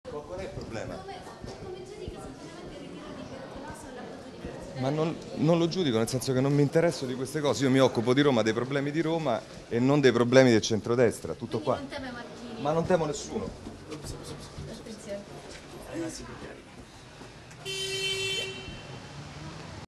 Giachetti: ritiro Bertolaso? Mi occupo di Roma no dei problemi del centrodestra, non temo nessu
(Agenzia VISTA) Roma, 28 aprile 2016
Il candidato PD a sindaco di Roma, Roberto Giachetti